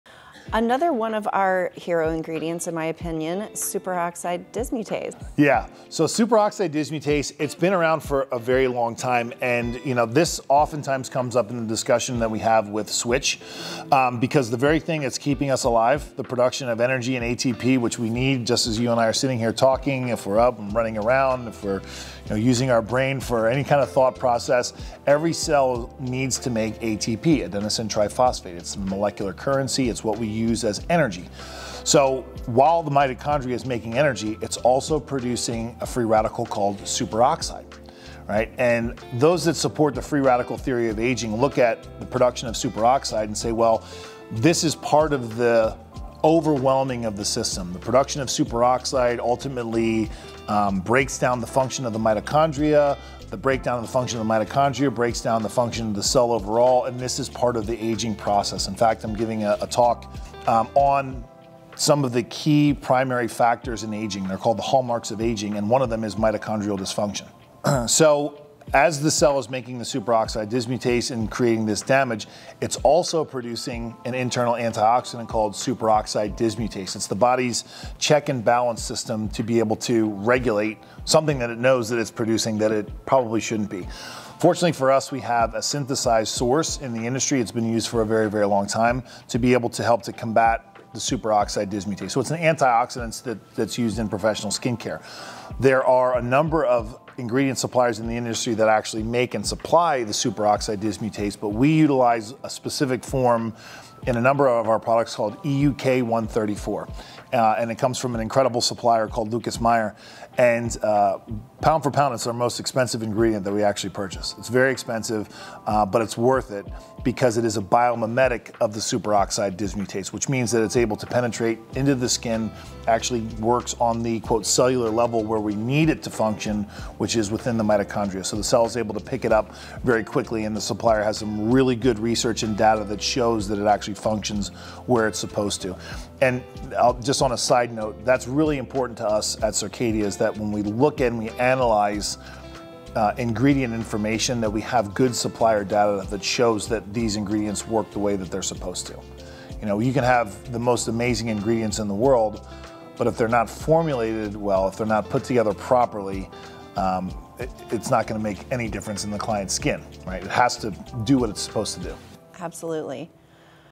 Superoxide Dismutase
In this discussion with Michael and Candice, they dive into the world of Superoxide Dismutase, a powerful antioxidant that holds the key to unlocking youthful, radiant skin.

From its role in mitochondrial function to its assistance with the aging process, the discussion will explore the science behind this essential molecule. Through the insights these industry experts give, you'll gain a comprehensive understanding of how superoxide influences cellular health and vitality.

But that's not all – Michael and Candice also shine a spotlight on a remarkable skincare ingredient: EUK 134. Sourced from the esteemed Lucas Meyer, this specialized form of superoxide dismutase represents the cutting edge of skincare innovation. Learn why EUK 134 stands out as a game-changer in professional skincare, thanks to its exceptional ability to penetrate the skin and combat oxidative stress.

Don't miss out on this informative discussion about Superoxide Dismutase and learn about the history and benefits of superoxide for yourself. Your skin will thank you!